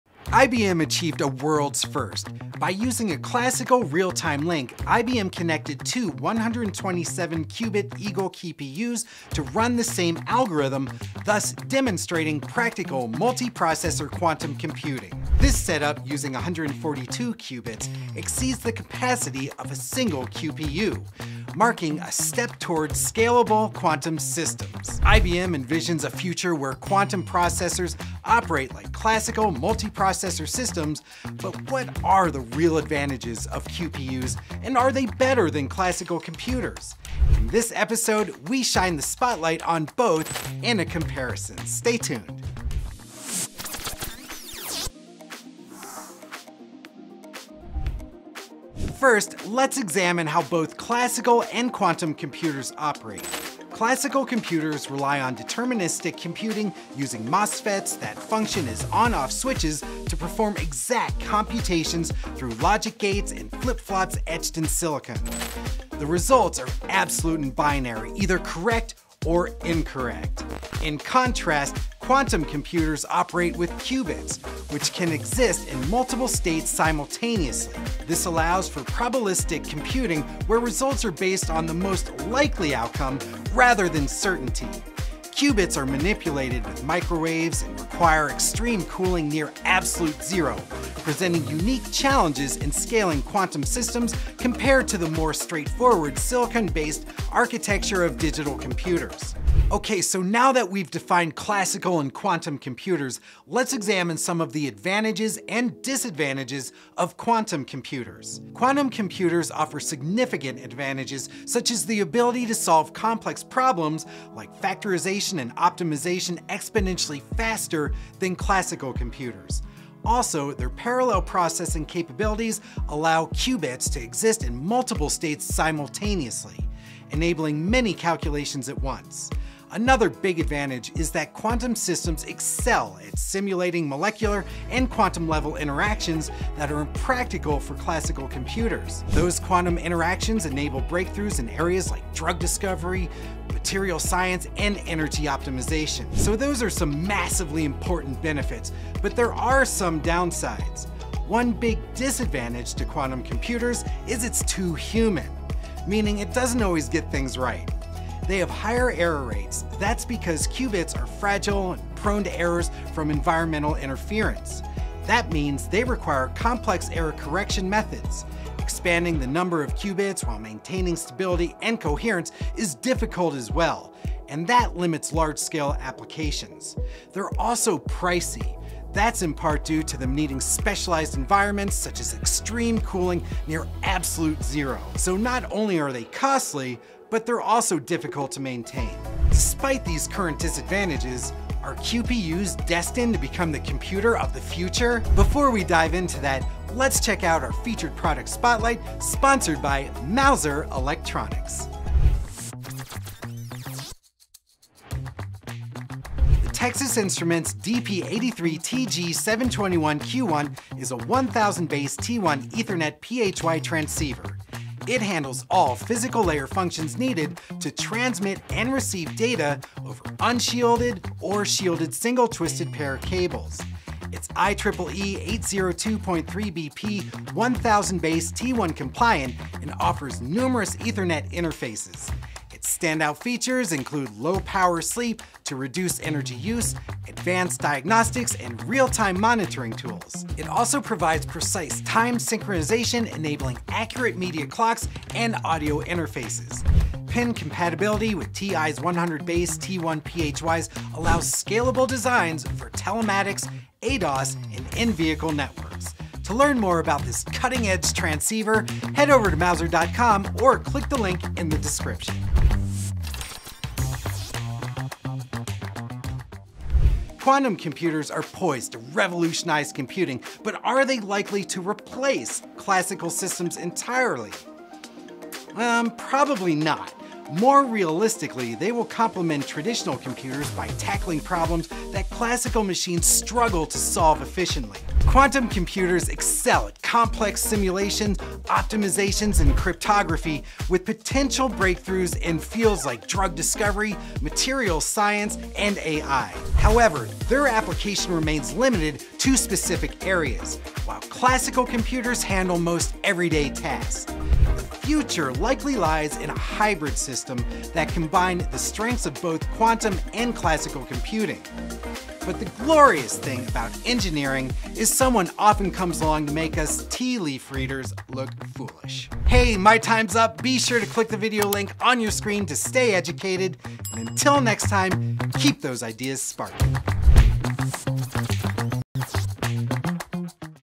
AAC Spotlight | Ep. 35 | Quantum vs. Classical Computing: Exploring the Differences
In this episode, we spotlight a comparison between classical computers and quantum computers exploring what they are, the pros and cons, and which one will likely win the future. Tune in to stay updated & educated on the latest in electrical engineering!

0:00 IBM achieves word first in quantum computing
0:43 How classical and quantum computers operate
1:48 The advantages and disadvantages of quantum computers
3:22 Featured Product Spotlight
4:27 Does the future belong to quantum computing?
5:19 Outro